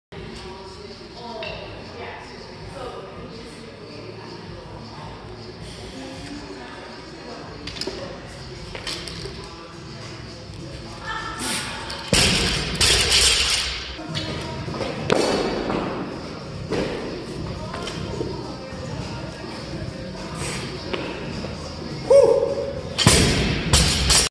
Clean Thrusters
As part of the workout today we worked up to a heavy Clean Thruster.

I've never actually tried to max this out before... It's kind of tricky!

The clean itself is pretty manageable, however, trying to turn that energy into a full overhead press is not very straightforward.

The first rep in the video was at 215 lbs and looked pretty clean. The second was at 245 and was a little sketchy... No need to press on beyond that. The risk of injury increases and the return on training investment really doesn't...